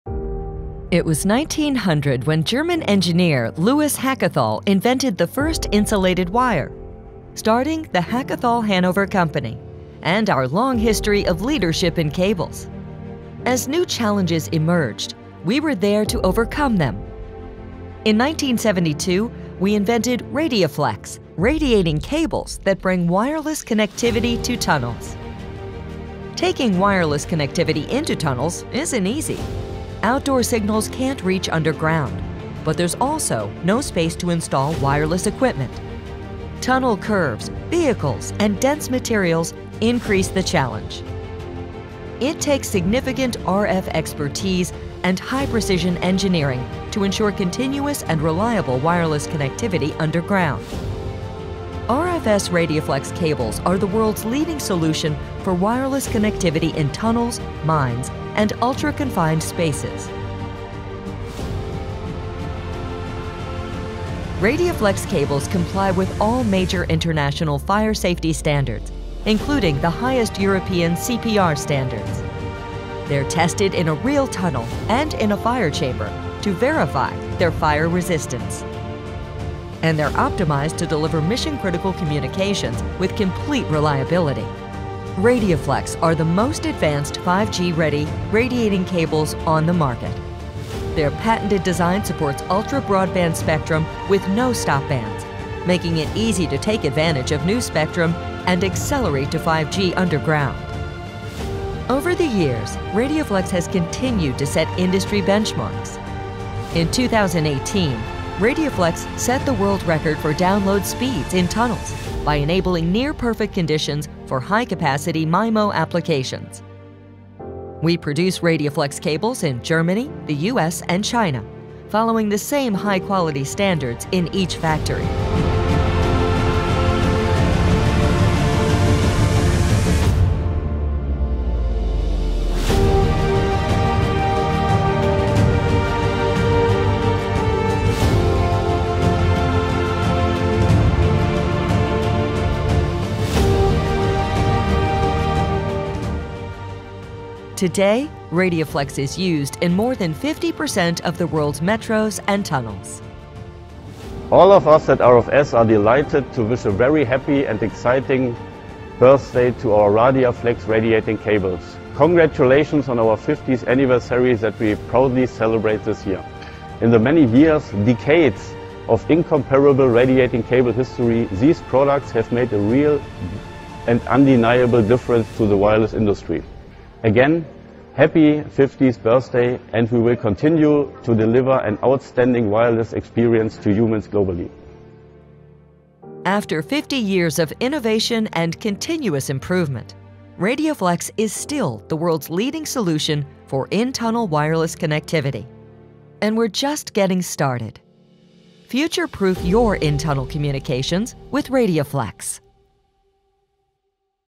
50 years of in-tunnel wireless connectivity with RFS RADIAFLEX® radiating cables
50 years ago, we invented RADIAFLEX® radiating cables to take wireless connectivity into tunnels. Today, RADIAFLEX cables are bringing 5G to tunnels and setting world-record speeds along the way. And they’re reliably delivering commercial and mission-critical communications in more than half of the world’s metros. Take a short trip into the world of RADIAFLEX to learn why this ingenious invention is still the world’s leading solution for in-tunnel wireless connectivity 50 years later. It’s been quite a journey so far, and we’re just getting started!

📢 For further information on RFS and our solutions/products, visit our